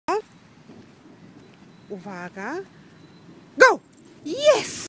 Szybkość
Jestem dumna z małego - zigonorawał dwa mega ulubione szarpaki i pobiegł tam gdzie chcę. To ćwiczenie robiliśmy pierwszy raz.